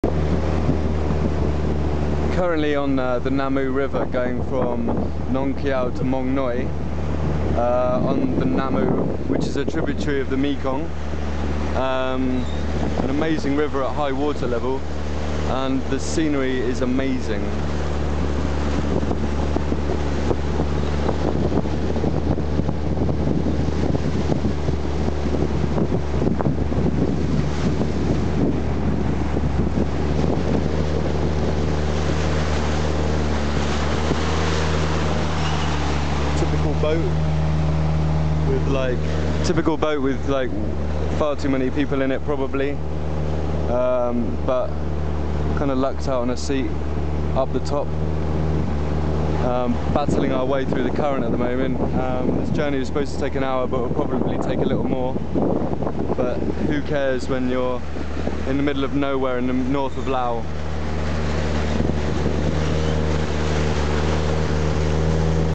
Boat up the Nam Ou- Nong Khiaw to Mong Noi
Travelling in Lao by boat from Nong Kiaw to Mong Noi up the Nam Ou river- a tributary to the Mekong. Amazing scenery, dense jungle and boat conditions you face on the same journey...